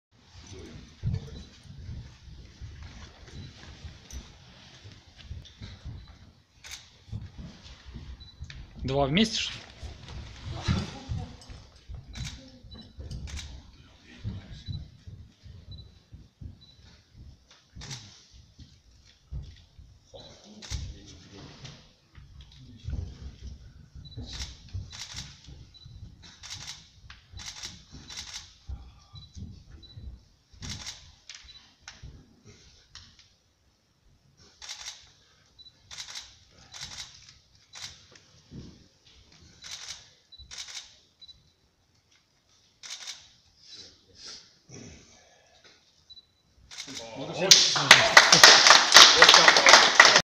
Sensei Kosorotov 8 Dan Judo / Seminar Yamashiro Sensei 9 Dan Uechi ryu karate
Budopass.com Загибание гвоздей Сэнсэй Сергей Косоротов 8 дан Дзюдо, Семинар Мастеров Уэчи рю каратэ с о.Окинава! Сэнсэй Ямаширо 9 дан, Сэнсэй Сусуму 7дан, Сэнсэй Минору 7 дан, провели семинар 6-7 апреля 2018 г.в Москве! seminar of Masters Uechi ryu karate from Okinawa! Sensei Yamashiro 9 dan, Sensei Susumu Kiyuna 7 dan, Sensei Minoru 7 dan, held a seminar on April 6-7, 2018 in Moscow!